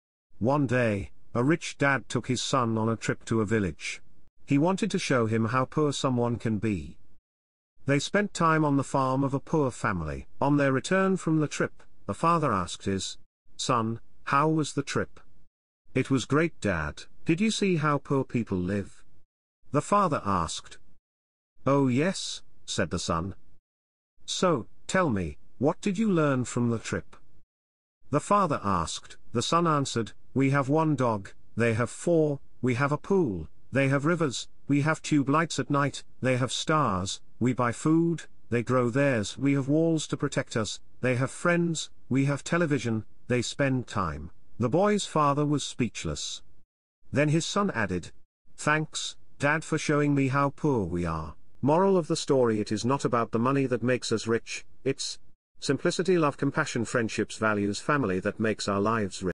Story with moral shorts # story # english short story
In this video you will learn English with the help of an interesting story. you will also learn a moral of the story.